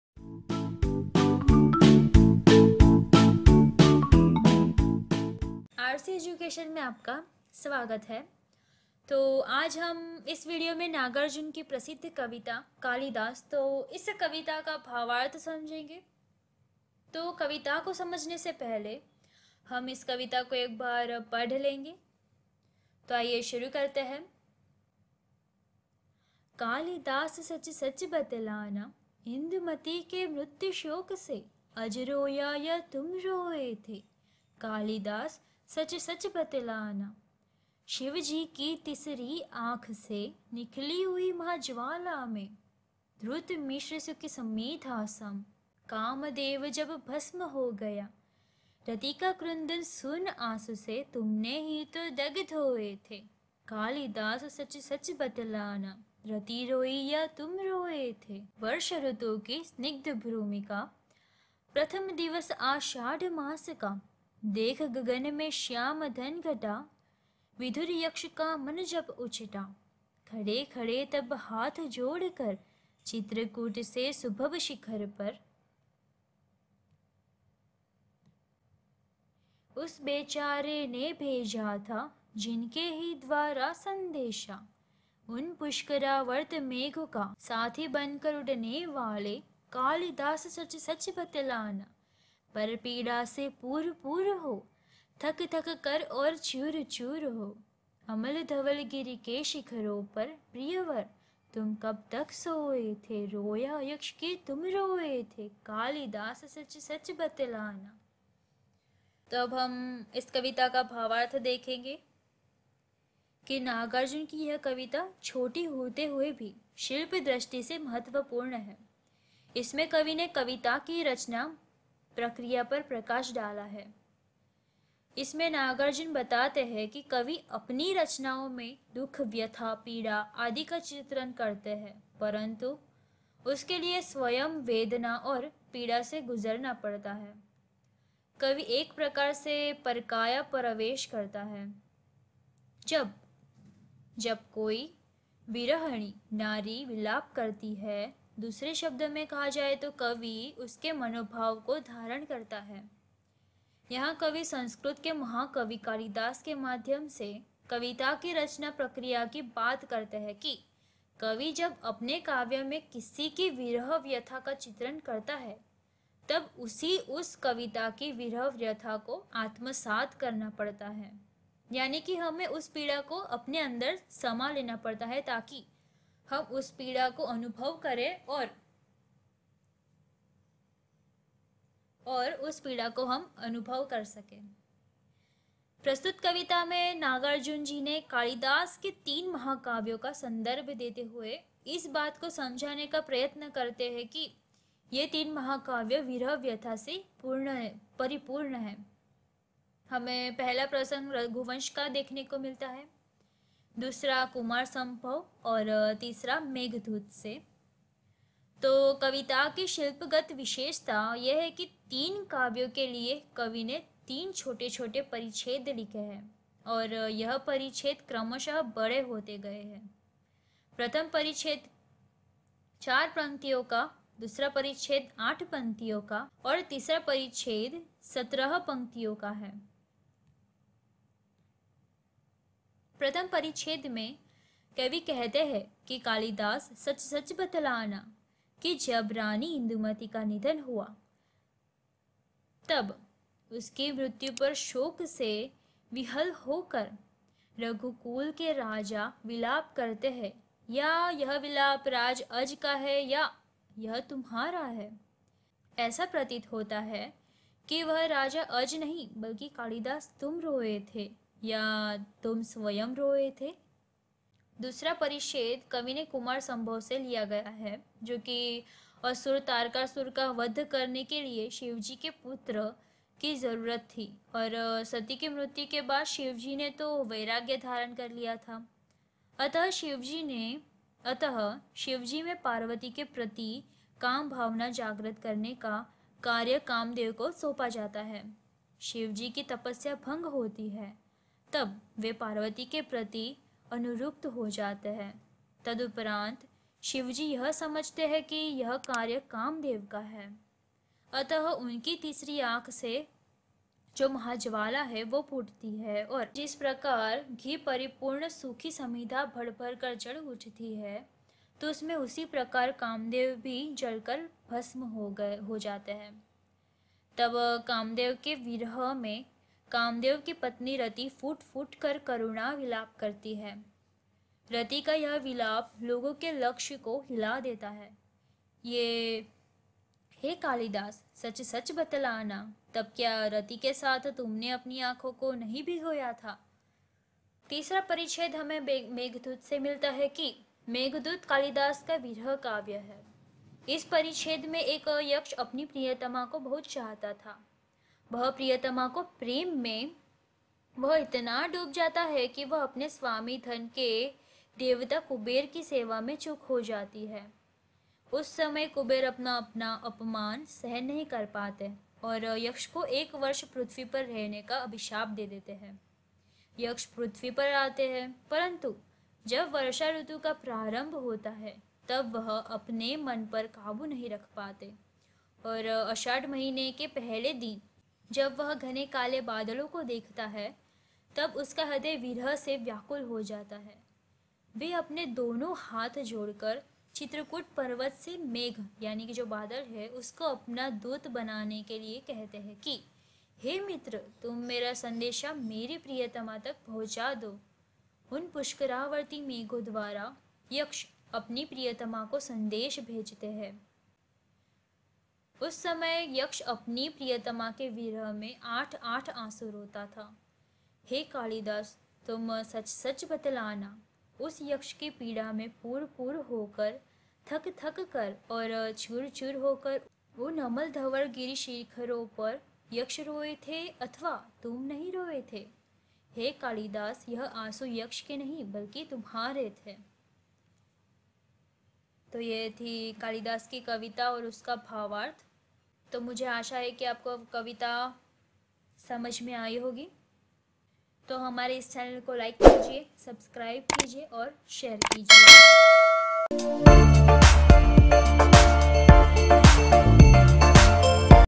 कालिदास कविता का भावार्थ || नागार्जुन \ Kalidas kavita explanation(B.A Hindi)
कालिदास कविता - नागार्जुन

RC Education मैं आपका स्वागत है| इस वीडियो में कालिदास कविता को सरल भाषा में समझाने का प्रयास किया है| कबीर दास भारतीय रहस्यवादी  कवि और  संत थे| हिंदी साहित्य के भक्ति काल के निर्गुण शाखा के महानतम कवि है| कबीर दास जी अपनी कविताओं में ज्यादातर राजस्थानी, पंजाबी, खड़ी बोली तथा ब्रजभाषा के शब्दों के उपयोग करते थे |

  कबीर दास से हमें अनेक रचनाएं देखने को मिलती है जैसे कि कबीर साखी, कबीर बीजक, कबीर दोहावली तथा कबीर ग्रंथावली|

इस वीडियो मेंप्रसिद्ध नागार्जुन ने कबीर दास से प्रश्न करते कविता दर्शाई है| कविता में हमें तीन प्रसंग देखने को मिलता है जोकि अलग-अलग कविता से लिए गए हैं| नागार्जुन ने कालिदास के ही कविताओं को उदाहरण स्वरूप दर्शाया है| इस कविता में नागार्जुन कालिदास से प्रश्न करते है की कालिदास दूसरों ने जो पीड़ा सही है वही पीड़ा आपने भी अपने अंदर कबीरदास जी ने समाई है|
इस कविता के माध्यम से कवि नागार्जुन ने यह समझाने का प्रयास किया है कि अगर किसी कवि को किसी और की पीड़ा के बारे में अपनी कविता में लिखना हो तो उसकी पीड़ा को अपने अंदर समाने  से या उस पीड़ा की अनुभूति करके हमें कविता लिखनी चाहिए|

धन्यवाद 🙏

Kalidas Poem - Nagarjuna

Welcome to RC Education. In this video, an attempt has been made to explain Kalida's poetry in simple language. Kabir Das Indian mystic Poet And was a saint. Hindi literature devotional times of the nirguna branch the greatest is a poet Kabir Das Ji in his poems mostly Rajasthani, Punjabi, standing bid, and Brajbhasha words Used to use

  Kabir Das from us Many creations can be seen As if Kabir Sakhi, Kabir Bijak, Kabir Dohawali, and Kabir Granthawali.

In this video the famous Nagarjuna wrote Kabir Das questioning the Poem is shown.us in poetry three episodes can be seen Which is Different taken from the poem. Nagarjuna used the poems of Kalidas only. for example, is shown. in this poem, Nagarjuna from Kalidas Question Does so that Kalidas the suffering that others have righted the same pain you too inside youKabirDas ji is contained.
through this poem, The poet Nagarjuna tried to explain that if a poet is about someone else's pain If you want to write a poem about the pain inside you in front of or feel that misery by doing We should write poetry.